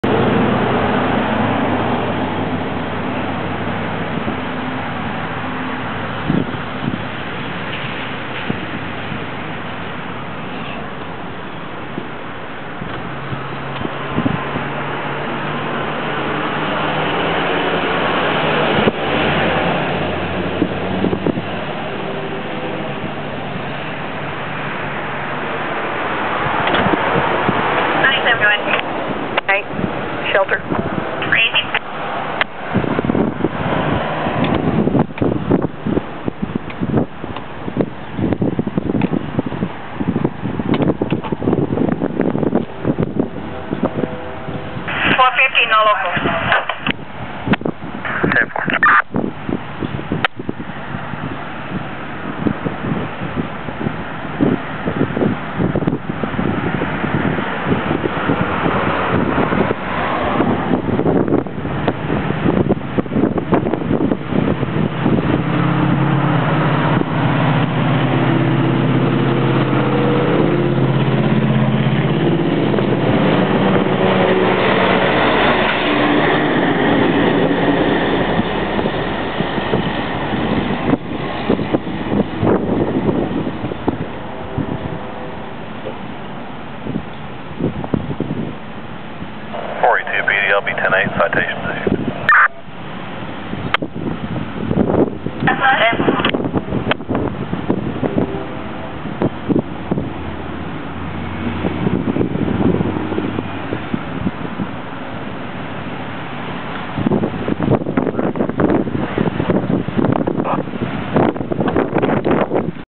Suspected DWI Palestine Texas
Listening to the VX6R and heard the officer going 10-6 across the street so I grabbed the camera but left the handheld inside. Missed the audio confirming the vehicle registered owner and driver's license/insurance status.  I did get the part where dispatch reports no outstanding warrants and the officer going 10-8 (back in service) with citations issued.  A traffic officer could be busy all day on Church street.